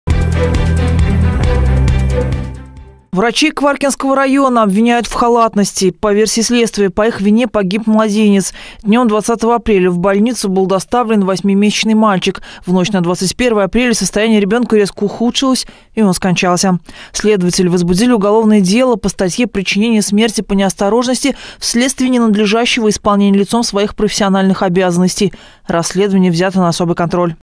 Врачей Кваркенского района обвиняют в халатности
По версии следствия, по их вине погиб младенец. Днем 20 апреля в больницу был доставлен 8-месячный мальчик. В ночь на 21-е апреля состояние ребёнка резко ухудшилось, и он скончался. Следователи возбудили уголовное дело по статье «Причинение смерти по неосторожности вследствие ненадлежащего исполнения лицом своих профессиональных обязанностей».